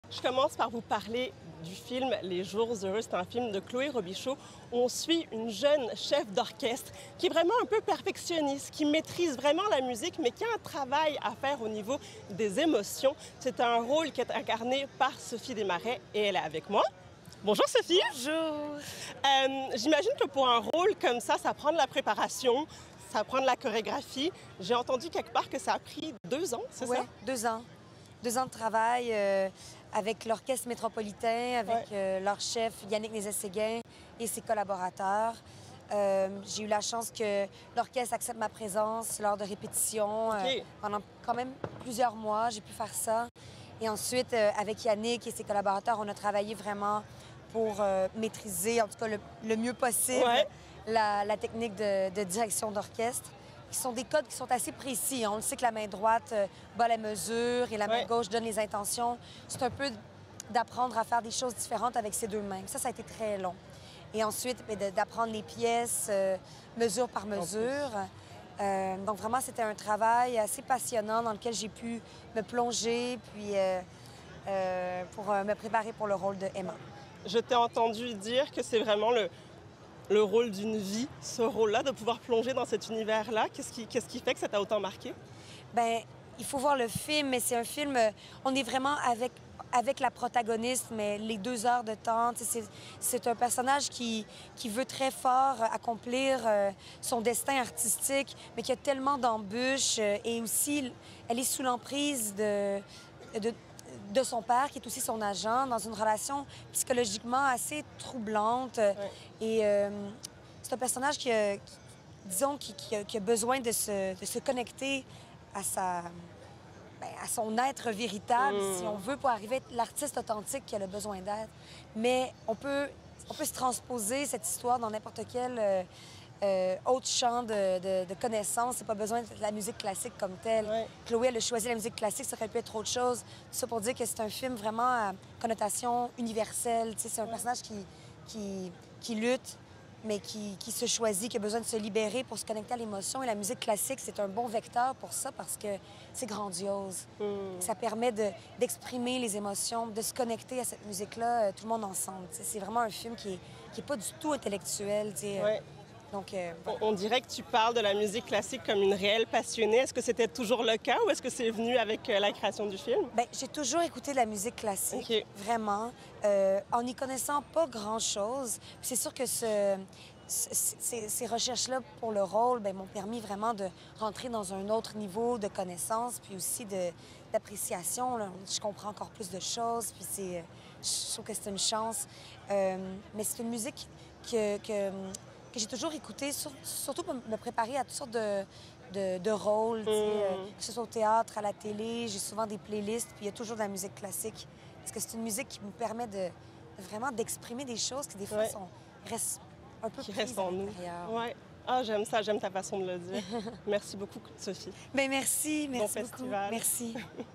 « Les jours heureux » : se plonger dans la peau d’une cheffe d’orchestre
L’actrice Sophie Desmarais s’est plongée dans la peau d’une cheffe d’orchestre au cours des deux dernières années pour son rôle dans le nouveau film de Chloé Robichaud Les jours heureux. En visite à Toronto, elle s’entretient avec Alison Vicrobeck dans le cadre du Festival international du film de Toronto.